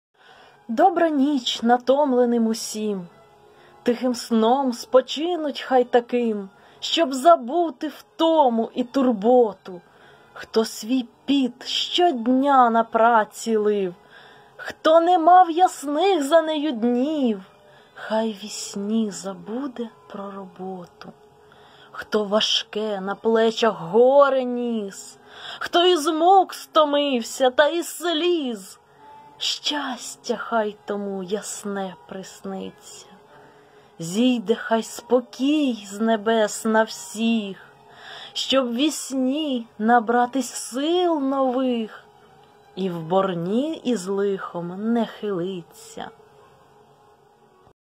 Борис Грінченко - На добраніч
Борис Дмитрович Грінче́нко (27 листопада [9 грудня] 1863, Вільховий Яр, нині селище Прелесне, Харківська губернія, Російська імперія — 23 квітня [6 травня] 1910, Оспедалетті, Королівство Італії) — український письменник, педагог, лексикограф, літературознавець, етнограф, історик, публіцист, громадсько-культурний діяч. Редактор низки українських періодичних видань, автор фундаментальних етнографічних, мовознавчих, літературознавчих, педагогічних праць, історичних нарисів, перших підручників з української мови й літератури, зокрема «Рідного слова» — книжки для читання в школі. Укладач чотиритомного тлумачного «Словаря української мови». Обстоював поширення української мови в школі та в установах. Літературні псевдоніми: Василь Чайченко, Л. Яворенко, П. Вартовий, Б. Вільховий, Перекотиполе, Гречаник. Один із організаторів і керівників «Просвіти», активний член Братства тарасівців, співзасновник Української радикальної партії (1904).